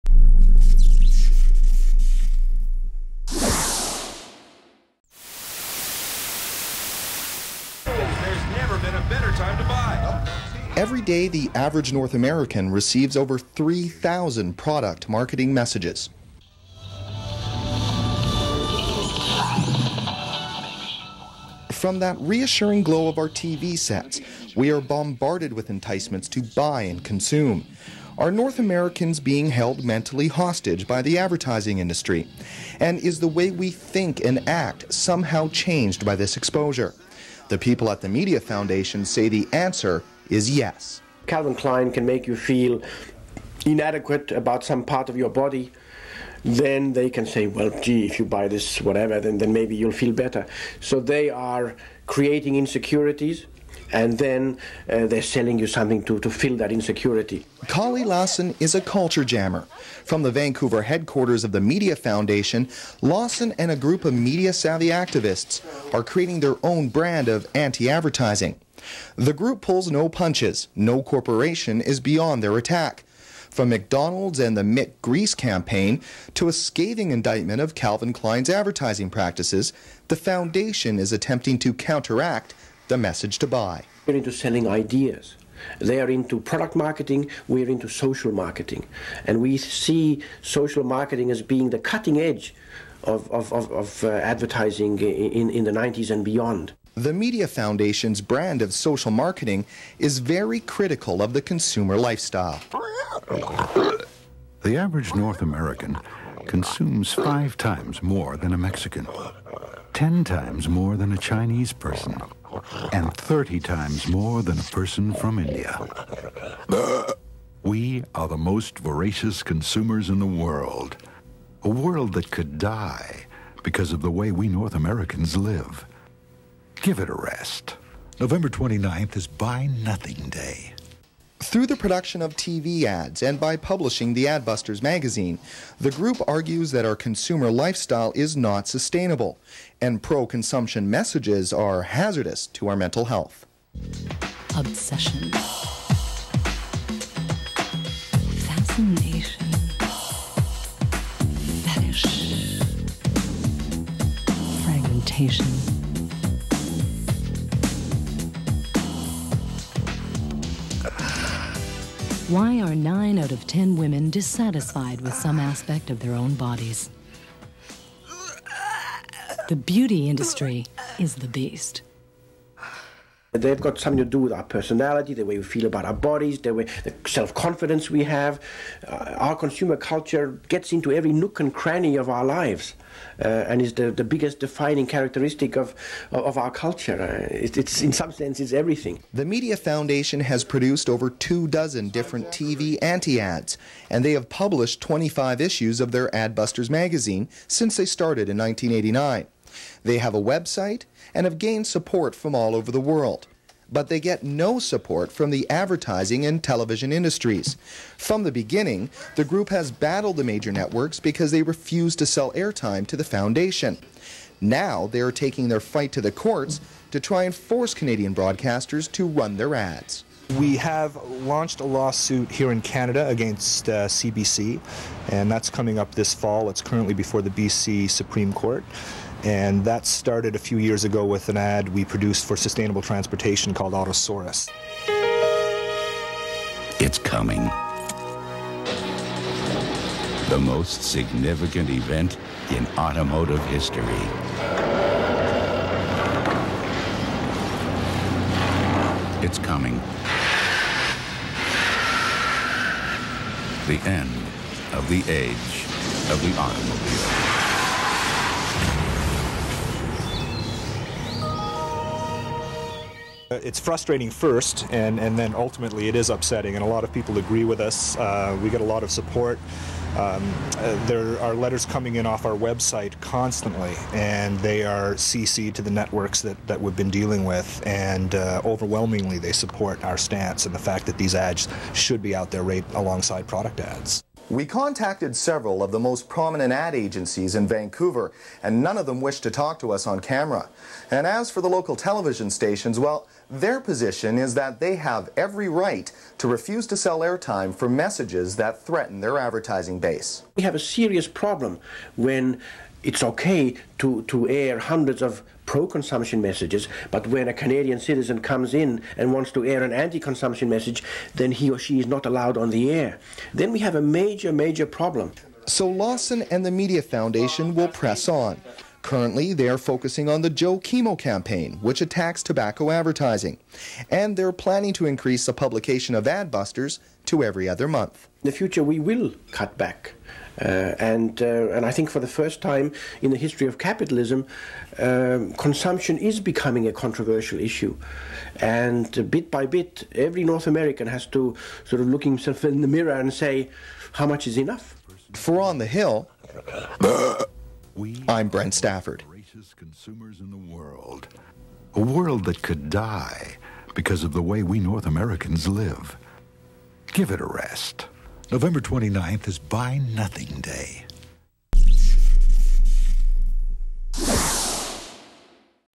Ad Busters-Culture Jamming (Sparked the Movement)
In 1997, Shakyegg produced one of the most comprehensive pieces on Kalle Lasn and the Ad Busters movement.  As you know Ad Busters and the Media Foundation is head quartered in Vancouver and are responsible for sparking the Occupy Wall Street Movement. This piece features some of Ad Busters' CLASSIC TV ANTI-ADS.

I am very proud of this piece and as you can imagine it takes an in-depth look at Ad Busters, their strategy and tactics as well as providing a strong critique of the advertising industry.  Its by understanding this critique from an academic POV which provides Shaky a core foundation for all the corporations client work.